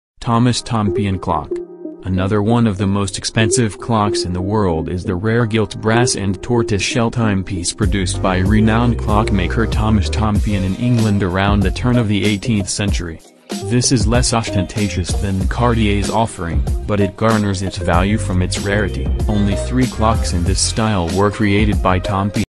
The World’s Most Expensive Clocks
Thomas Tompion Clock
Another one of the most expensive clocks in the world is the rare gilt brass and tortoiseshell timepiece produced by renowned clockmaker Thomas Tompion in England around the turn of the 18th century. This is less ostentatious than Cartier’s offering, but it garners its value from its rarity – only 3 clocks in this style were created by Tompion and it sold at auction for $2m in 1999. If it comes back on to the market again, it might well jump further up this list.